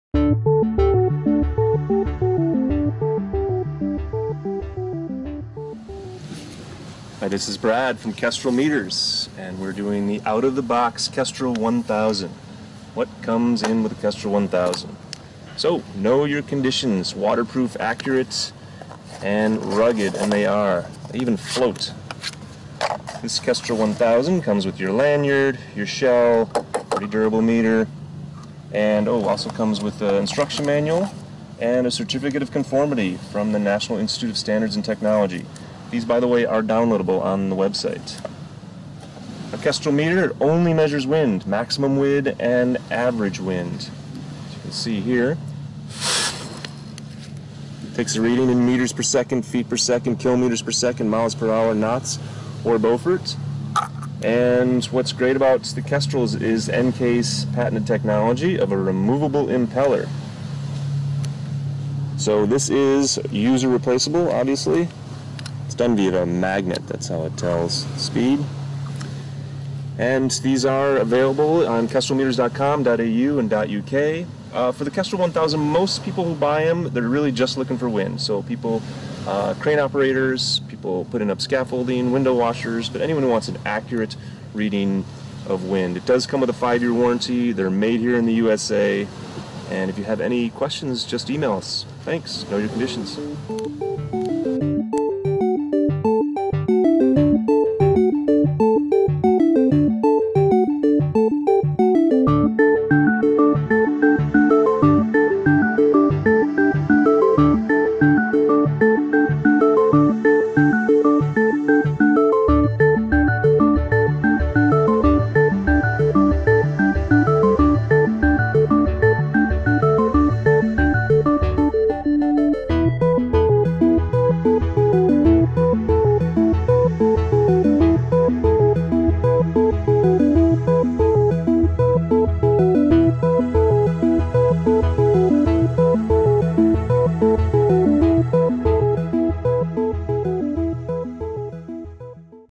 Kestrel 1000 Unboxing (International) - KestrelMeters.com
Watch Brad from KestrelMeters.com unbox and talk about the Kestrel 1000 Portable Professional Weather Meter.  Know Your Conditions.